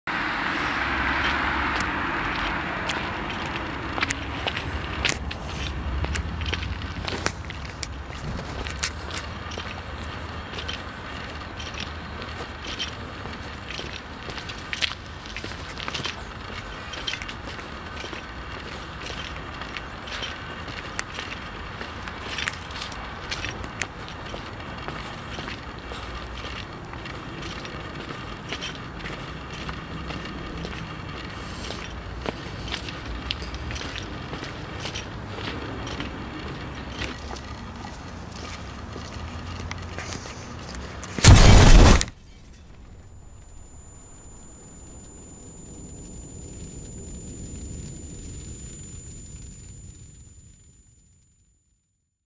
Spectacle Artefact (2010)
A quick explosion test for Coventry University.

Shot using Canon 5D Mark II